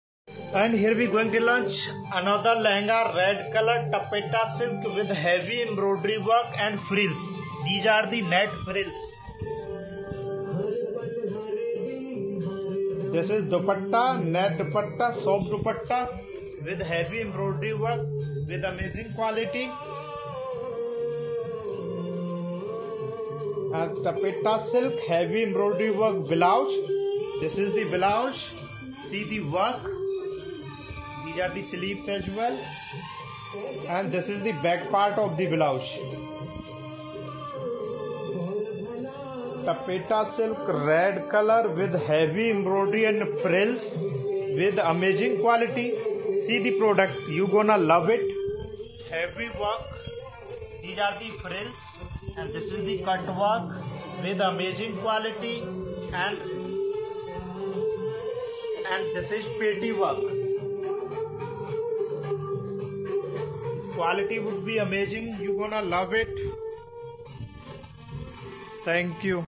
price 1100
*Sale sale sale sale sale*

Sale Rate:-   1099 only

* BD Launching New Designer Taffeta Silk embroidery work Lehenga*

*BD-1096*

*FABRIC DETAILS*
*👗*LEHENGA*👗*
*LAHENGA FABRICS: Taffeta Silk Heavy Embroidery Frill Work Lehenga*

*👗*CHOLI*👗*
 *CHOLI  80 cm : Taffta Silk heavy embroidery work*

*👗*DUPATTA*👗*
*DUPATTA 2 METER  : NET WITH HEAVY EMBROIDERY WORK*

*LEHENGA LENGTH=42*

*Size = Up to 44*

 👆🏻 *BEST QUALITY BEST PRICE* 👌🏻